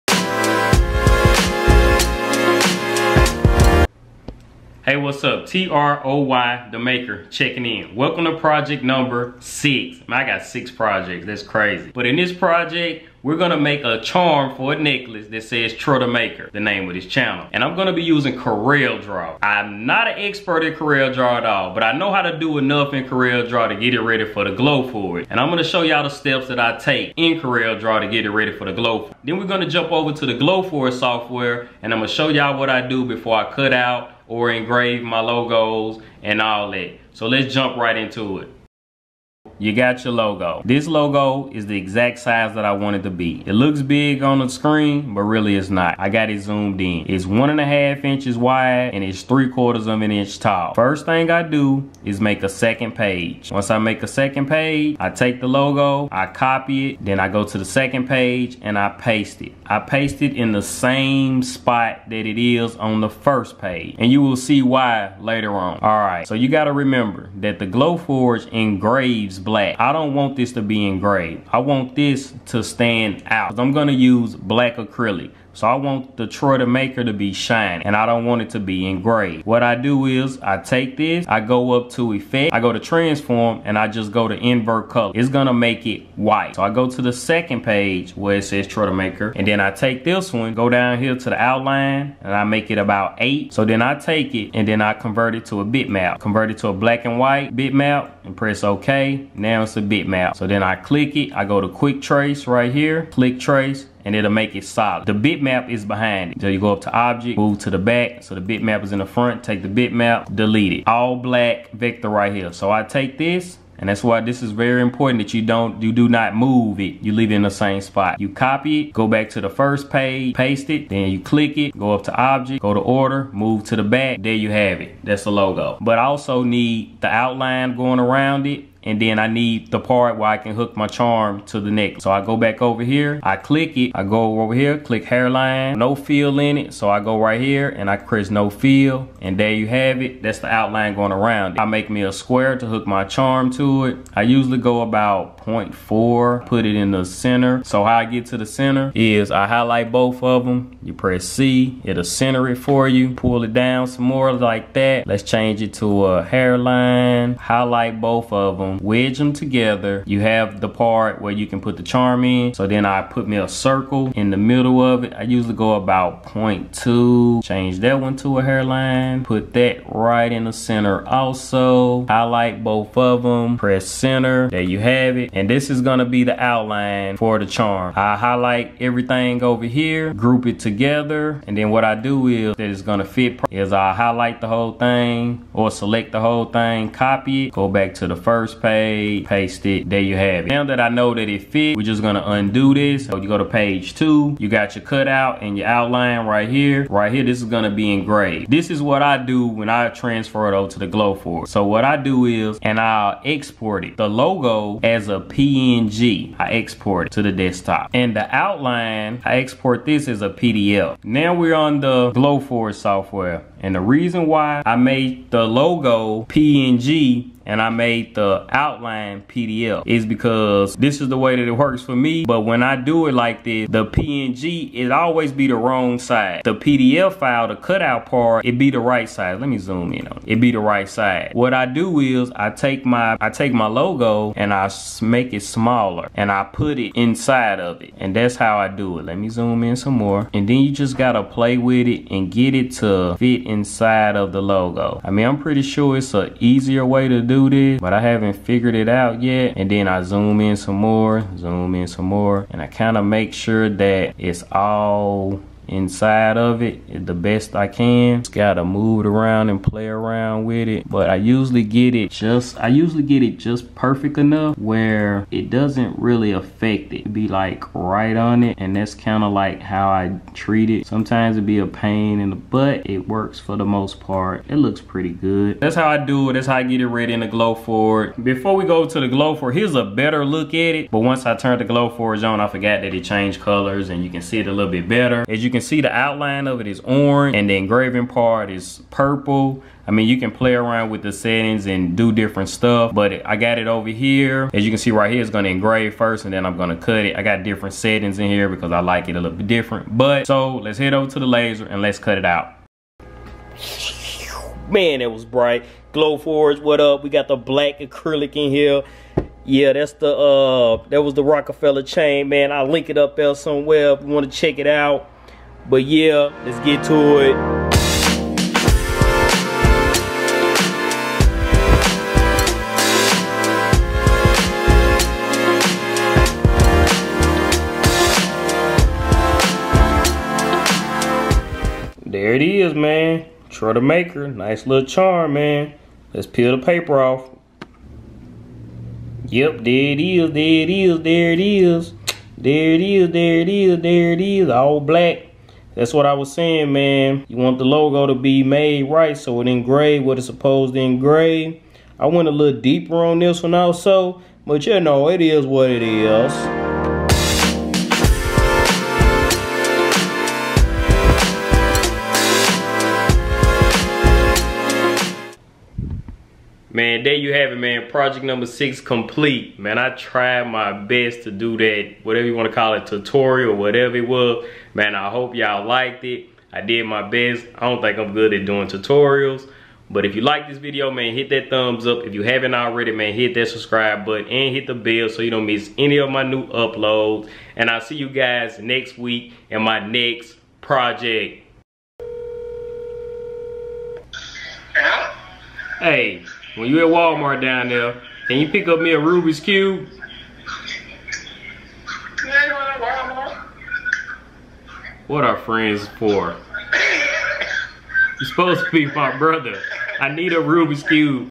Laser cut jewelry tutorial using Glowforge and Coreldraw.
This video project is a tutorial on how I design my jewelry using coreldraw and a glowforge 3d laser printer.